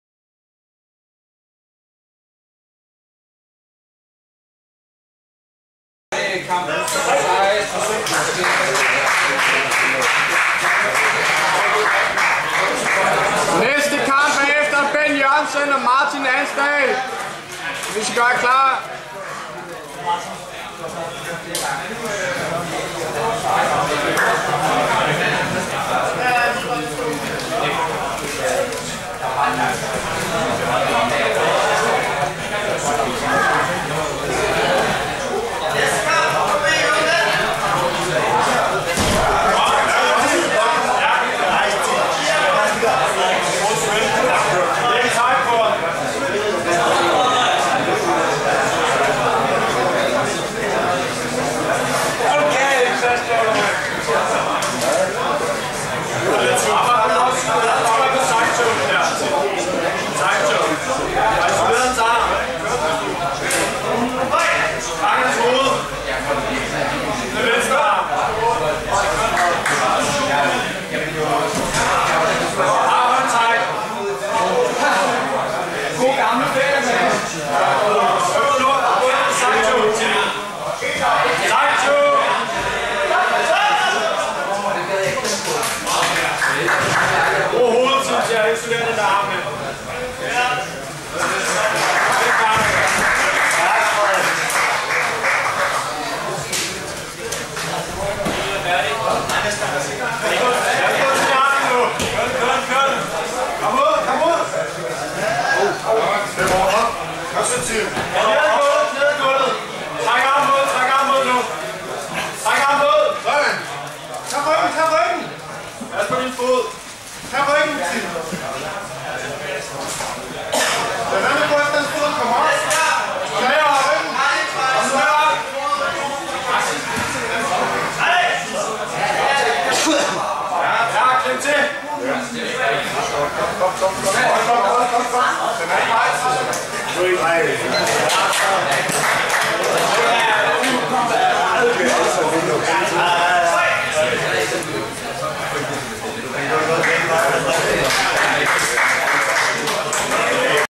DGL 3 Runde 2015 Tim Vuong CheckMat/Arte Suave vs Frederik Ruth Mikenta
Dansk Grappling Liga 2015 Tredje Runde Sjælland
Gruppe A Øved Mænd -77.0 kg

 Ben Jørgensen SIAM vs Rico Birch CheckMat/Arte SuaveTim Vuong CheckMat/Arte Suave vs Frederik Ruth Mikenta